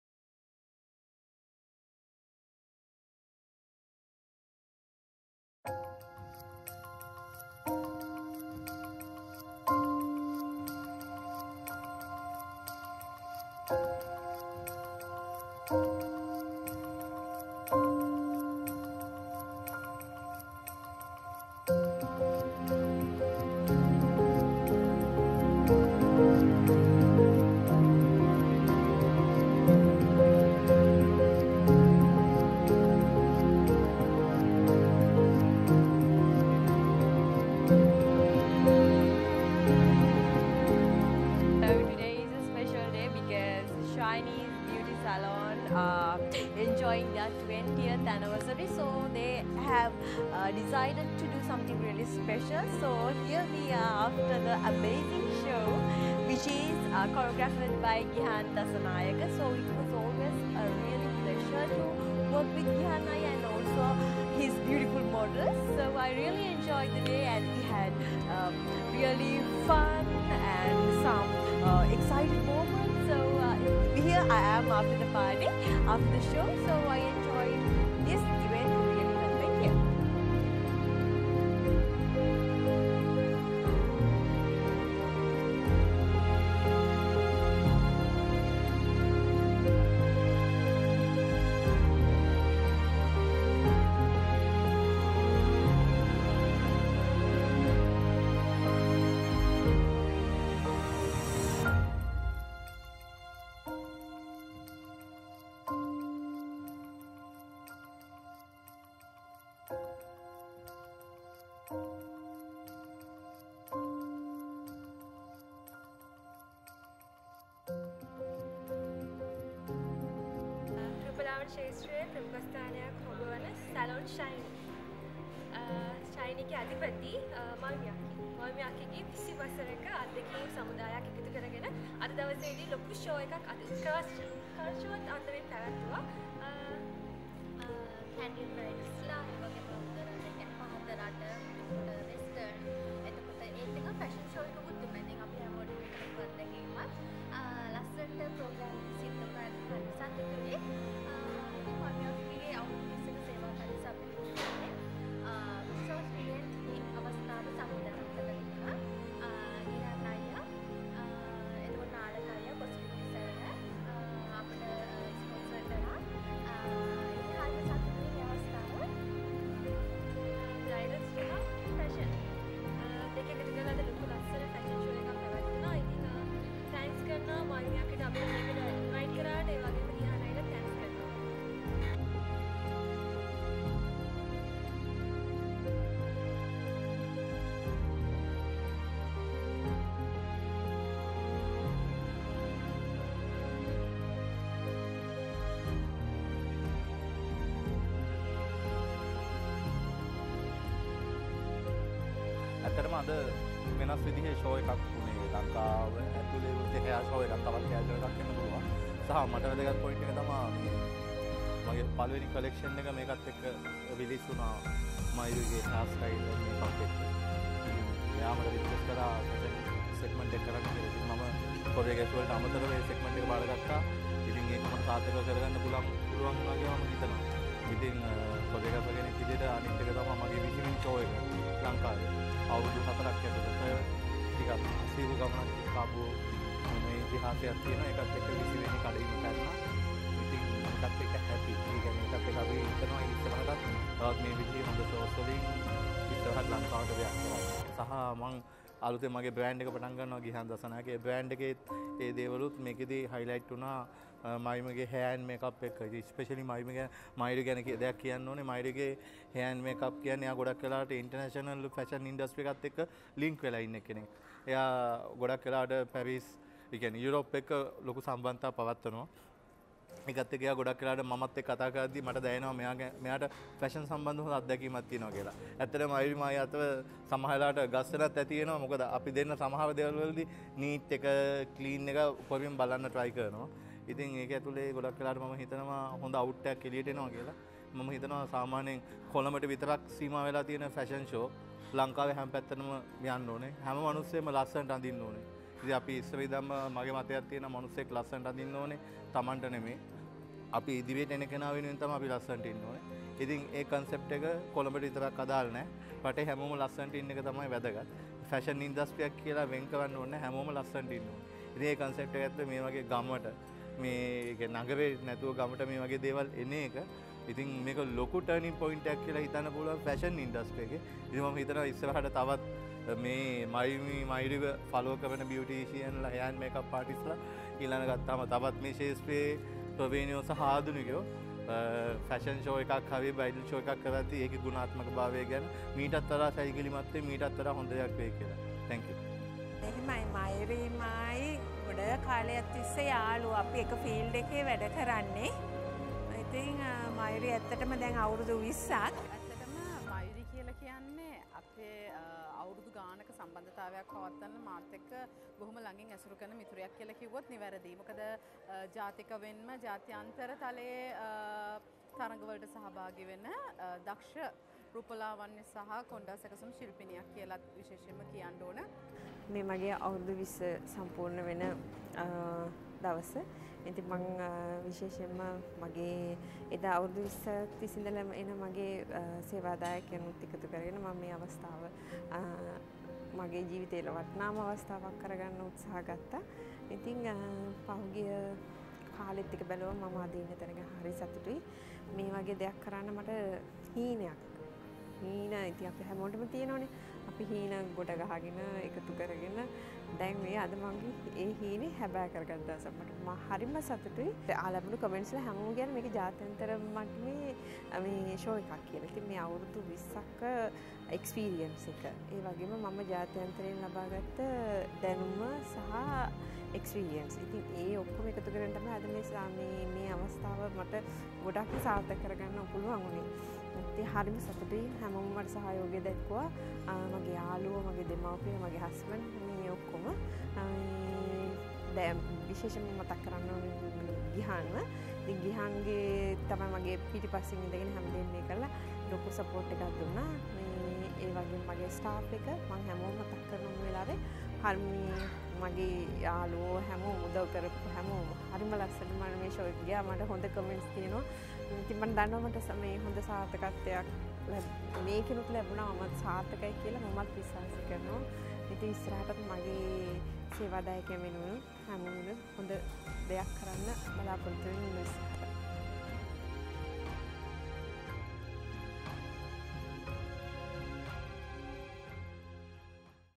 SALON SHINY 20TH ANNIVERSARY (2019) with Fashion Segment voice
SALON SHINY 20TH ANNIVERSARY (2019) with Fashion Segment
HAIR AND MAKEUP RUNWAY 2019
HAIR AND MAKEUP BY SALON SHINY
Photography by Eranga Sampath
Choreography by Gihan Dassanayake
Fashion costume designing by
Gihan Dassanayake
Videography by Arora video production